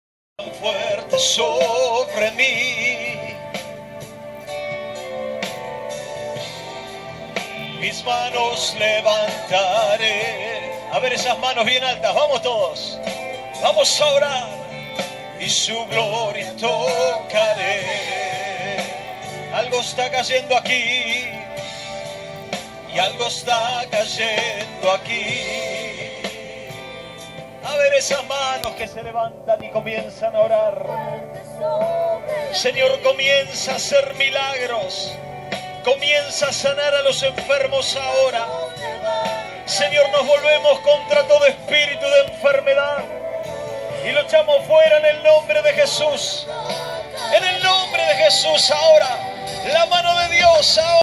ANTONIO-MAR DEL PLATA - JESÚS TE AMA(2)
Campaña Evangelística de Carlos Annacondia, 26/11/2016.